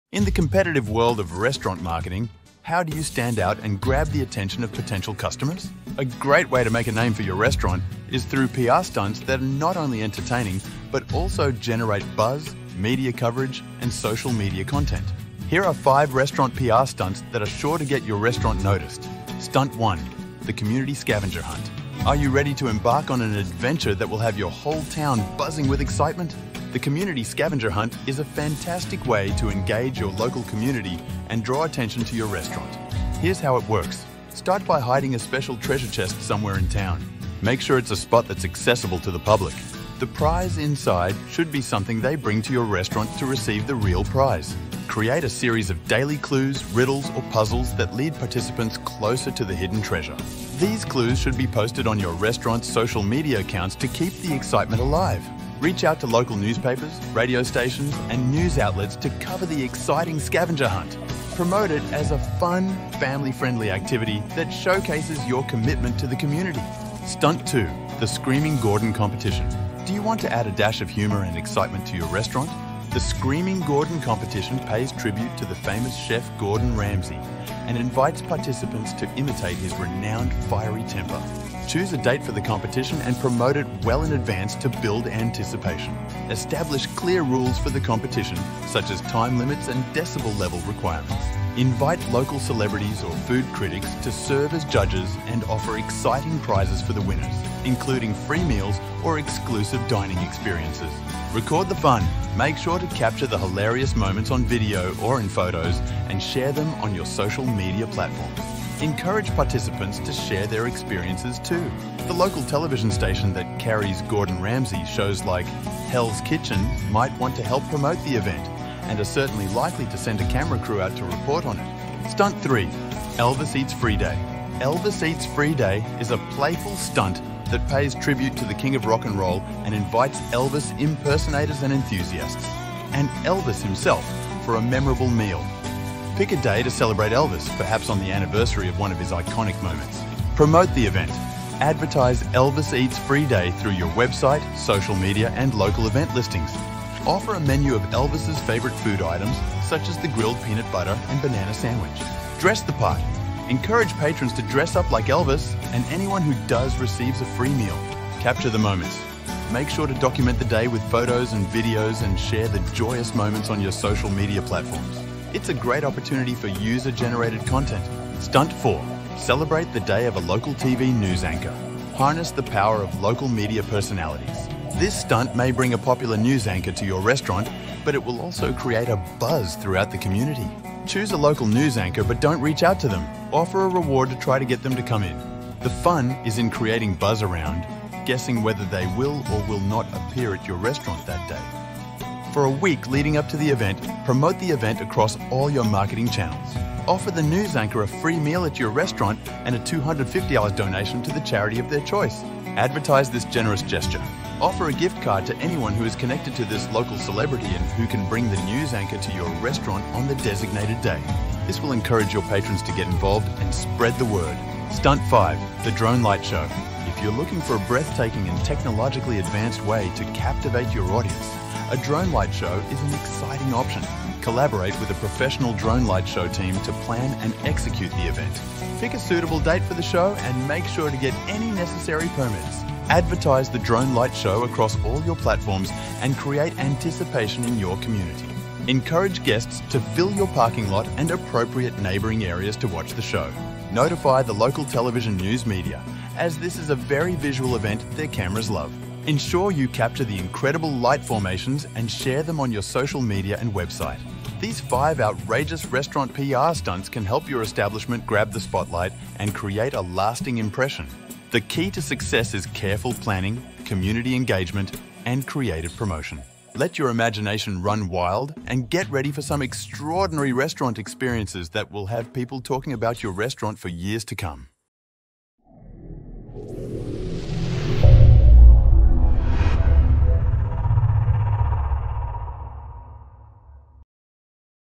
5 Outrageous Restaurant PR Stunts That Guarantee News Coverage
If you’re looking for creative and outrageous ideas to promote your restaurant, we’ve got you covered. Here are five restaurant PR stunts that are sure to get your restaurant noticed.